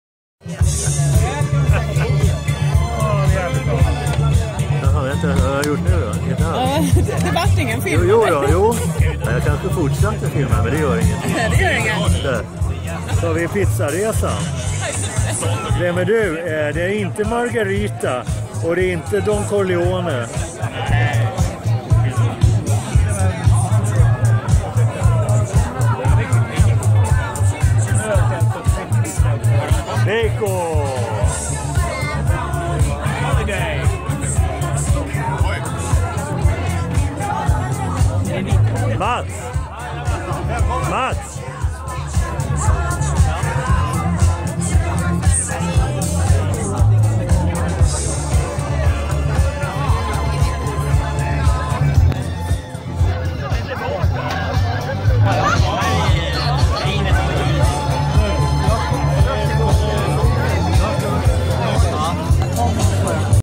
PizzaPartySthlm@seaside
Having a snack in between dancin´´ to keep the body energy-level up to par with the mental level, which already exeeds +100%...